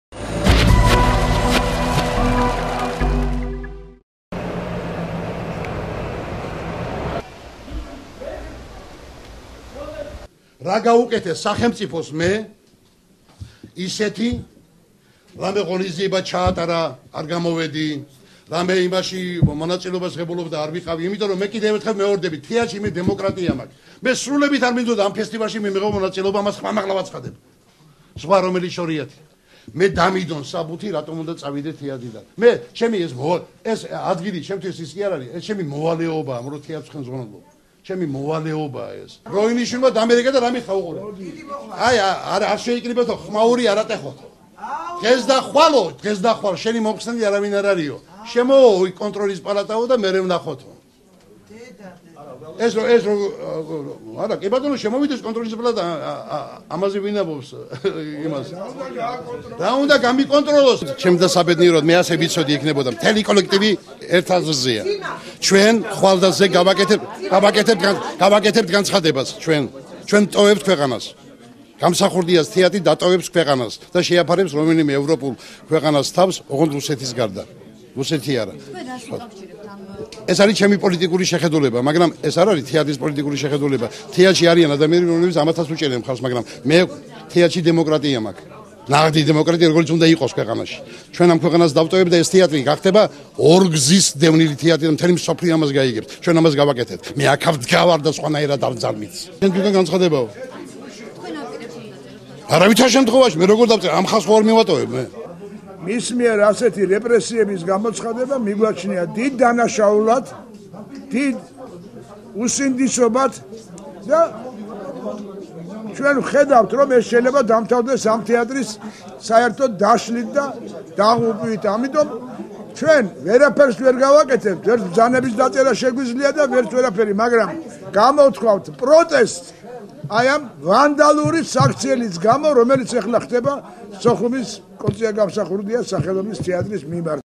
26.10.09
სოხუმის თეატრის სამხატვრო ხელმძღვანელი დიმა ჯაიანი შესაძლოა თანამდებობიდან გაათავისუფლონ